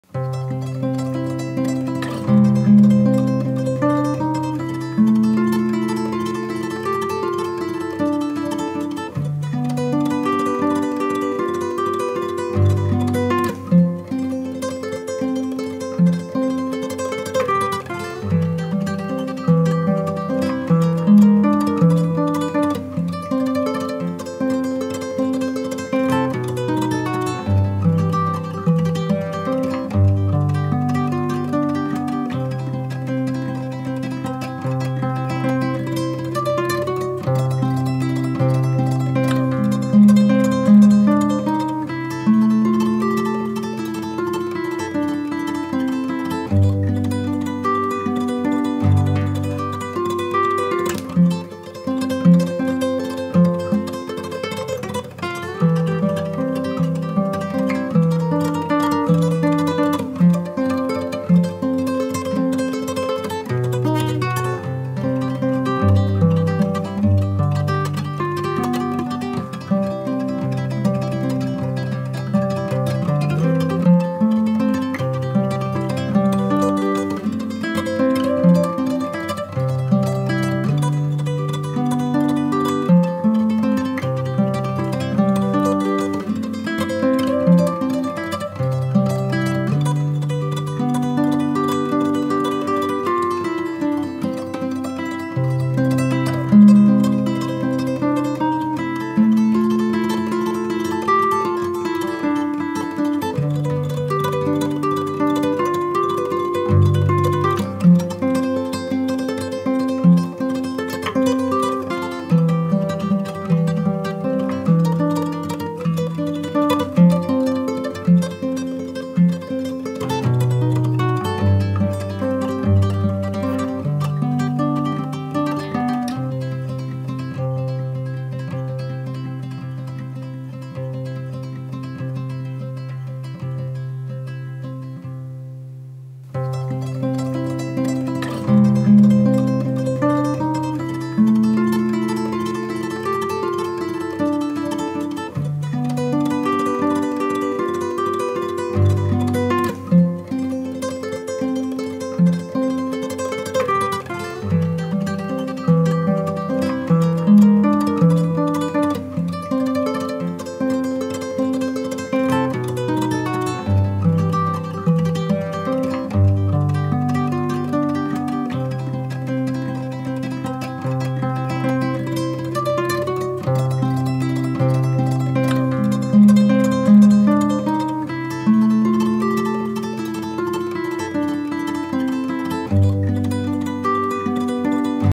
Satisfying Valentine's Day Painting
An abstract heart painting made with valentine's day colors.